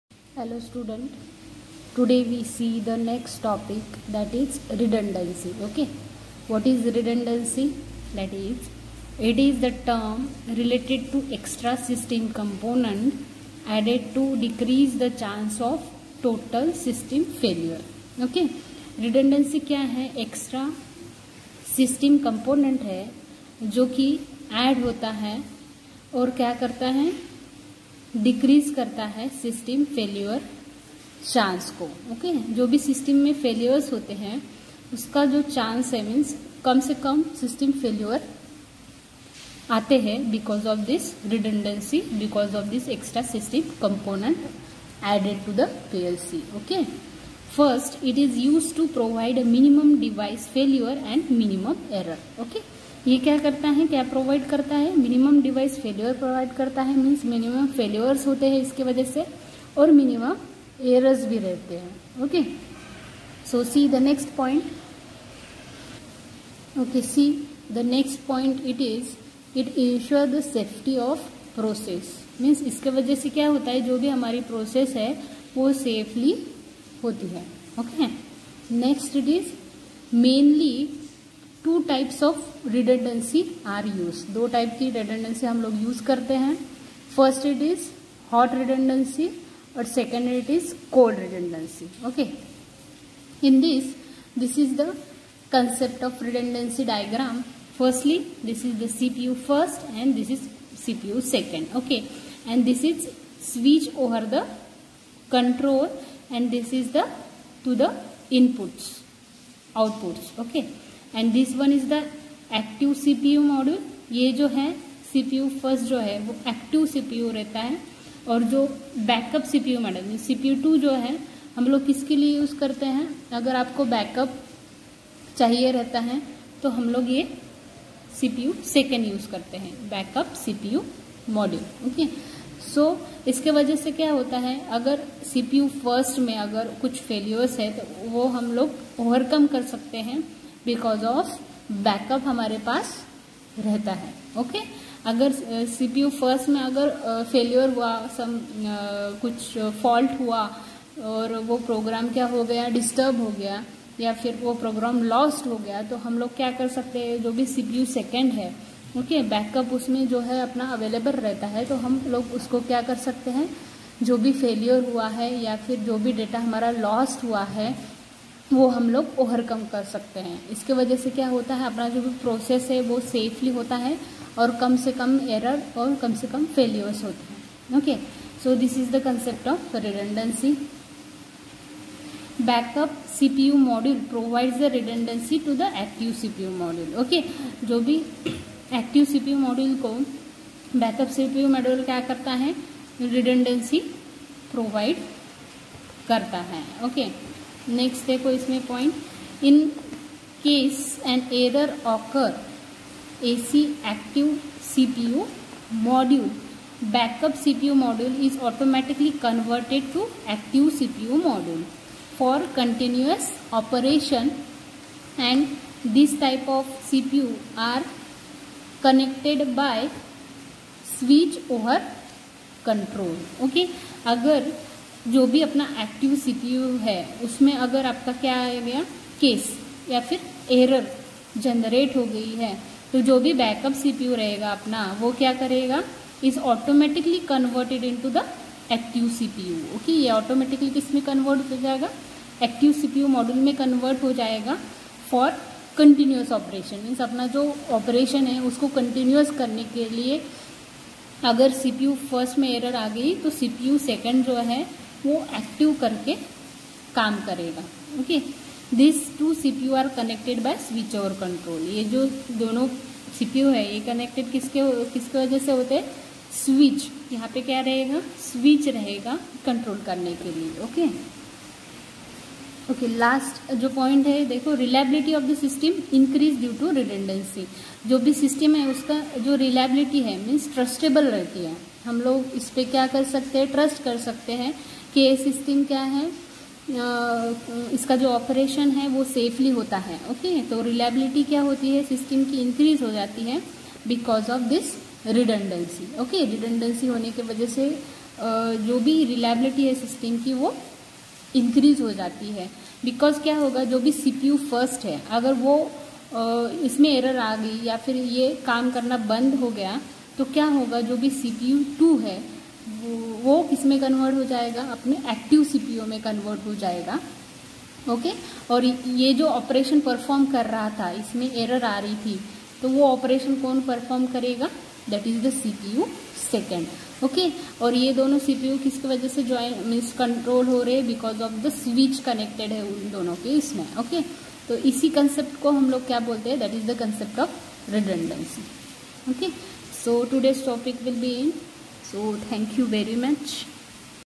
Concept of redundancy in PLC | Lecture 8
This video lecture is useful for diploma in electronics and telecommunication Students [Subject:- Industrial Automation  ( IAU)].This video lecture cover following points.
1)Concept of redundancy in PLC